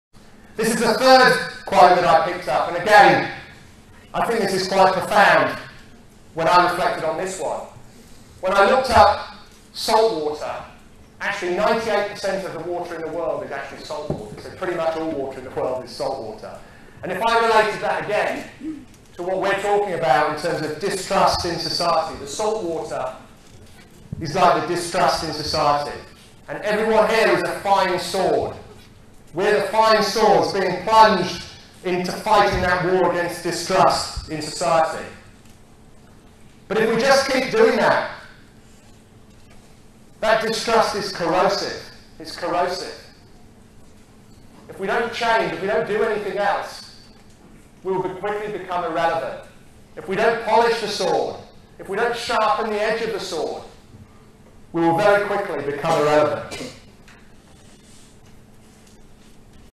Art of war - Even the finest sword plunged into salt water will eventually rust
Jim Woods sharing his 3 favourite quotes from Art of War  and how they apply to our working environment.